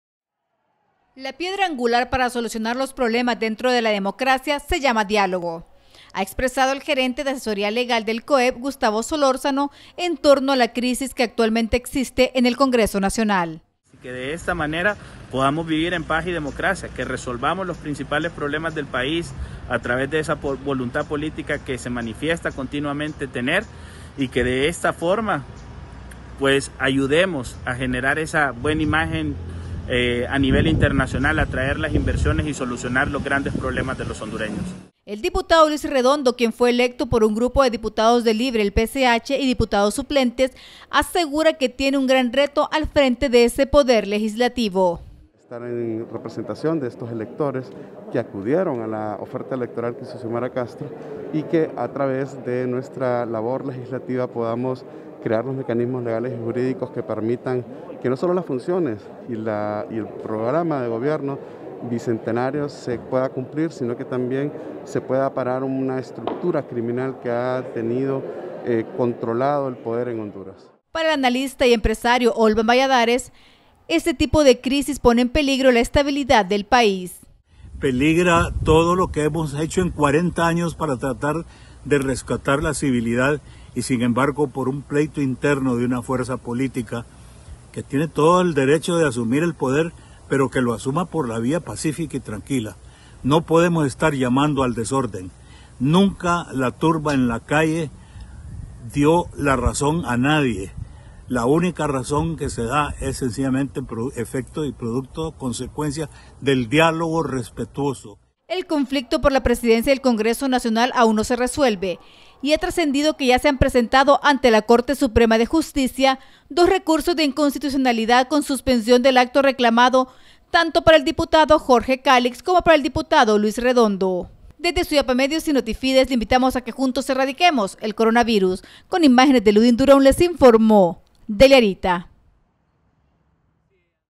Empresarios exigen diálogo
Empresarios se muestran preocupados por la crisis del Congreso Nacional, porque según ellos, esto aleja a los inversionistas y no favorece un clima para estas transacciones.